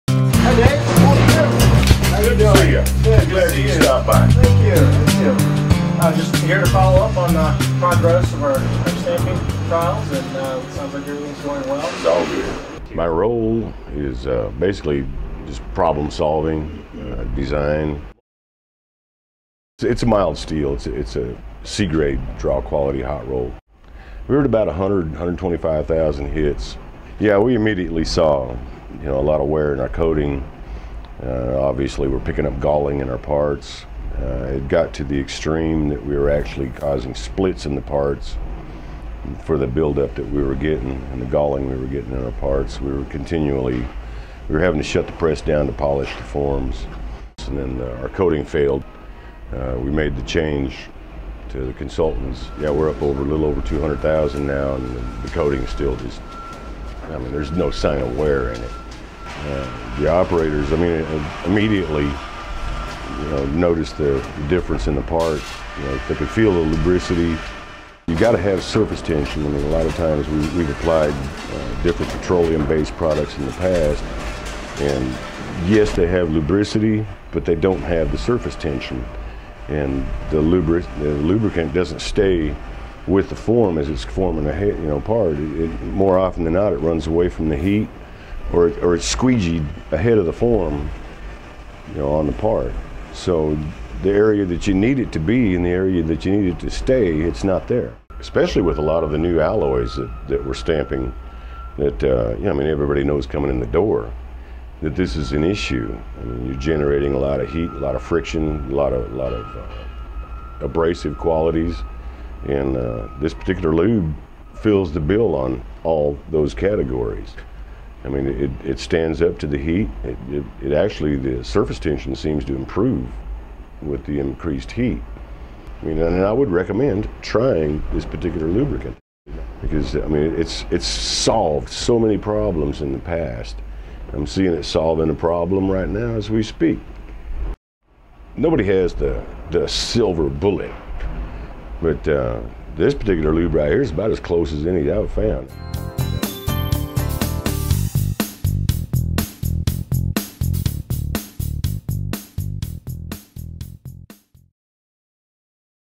CLI3900 Stamping Lubricant Extending Tool & Die Life
Demonstrating amazing results of CLI 3900 Stamping Lubricant by Consultant Lubricants by extending tool and die life and maximizing surface tension when drawing parts.